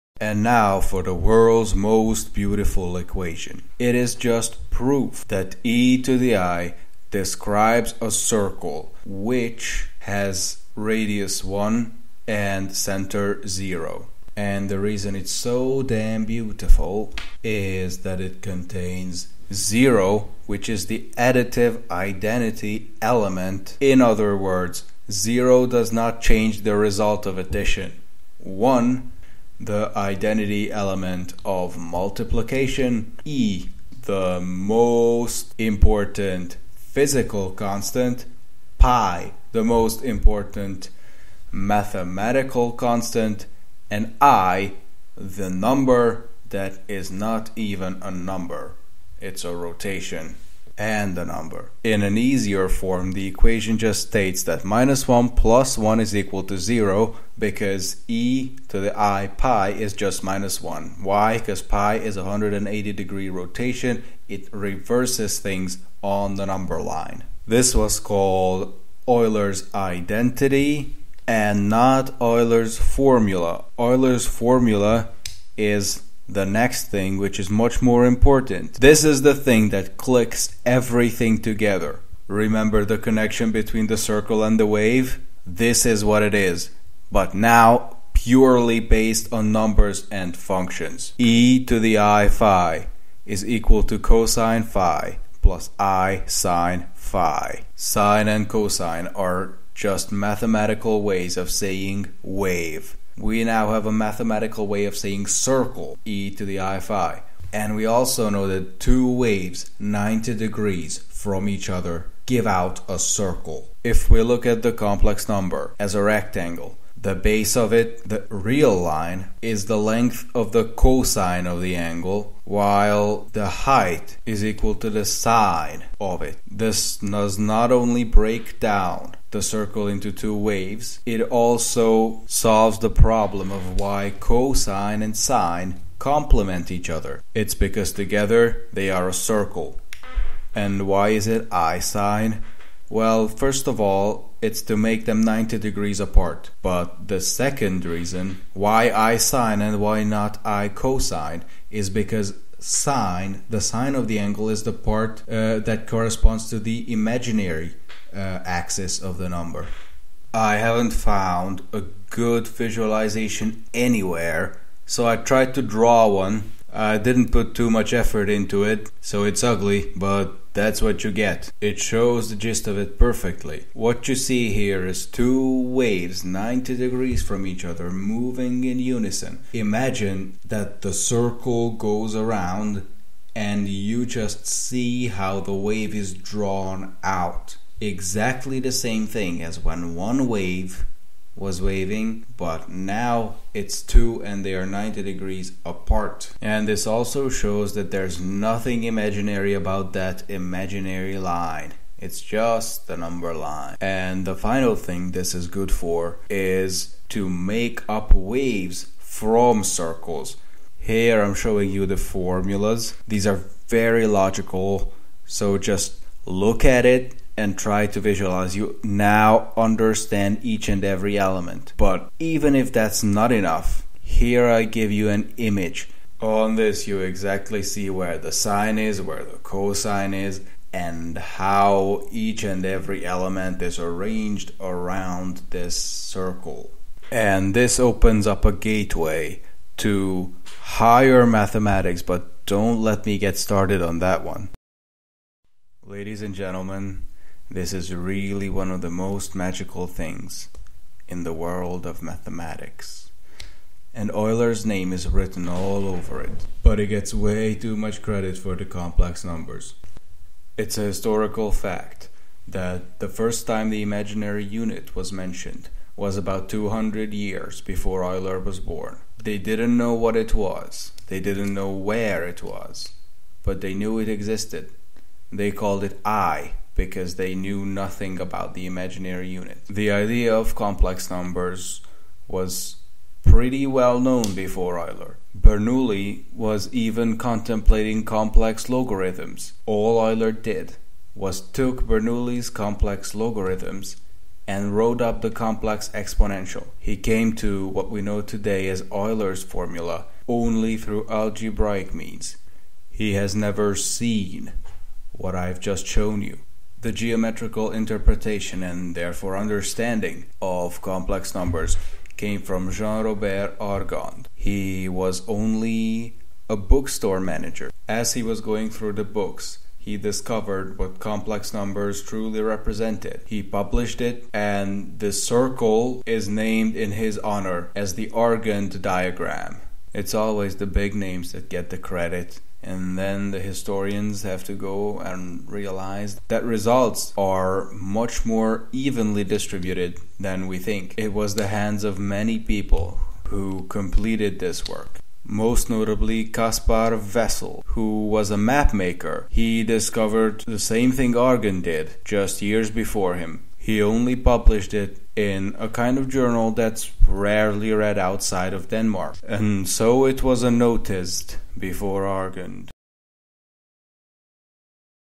Euler didn't invent complex numbers!
This video is an excerpt from the lecture titled "The Cycle". The lecture takes one from simple high school trigonometry to complex numbers and phasors.